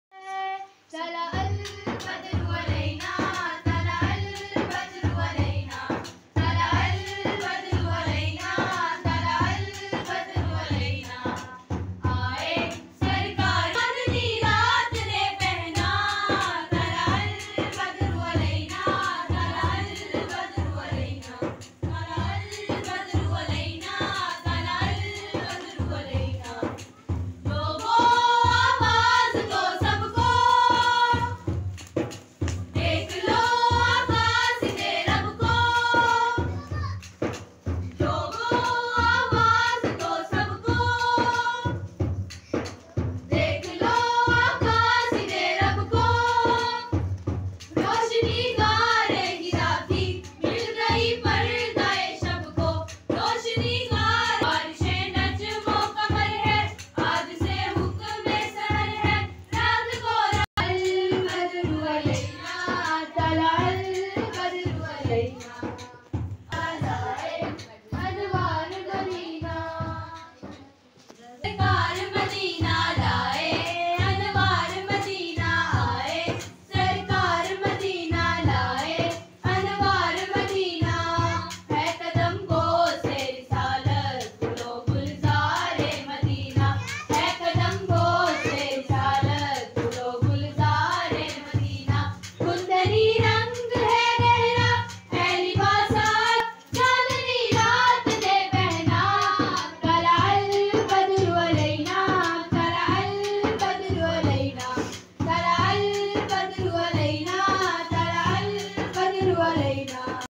Naat e Rasool Maqbool..Eid Milad Un Nabi Celebration by Montessori Section..Shadman Campus
Video from The Exaa School System